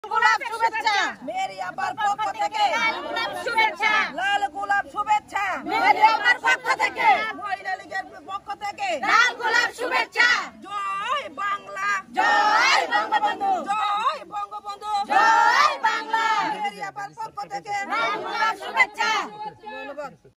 Short news Videol mohila Awami League/titas upojela।তিতাস উপজেলা মহিলা আওয়ামীলীগ।@stvnews14
তিতাসে মহিলা আ:লীগের ৫৪ তম প্রতিষ্ঠা বার্ষিকী পালিত -

বাংলাদেশ আওয়ামী লীগের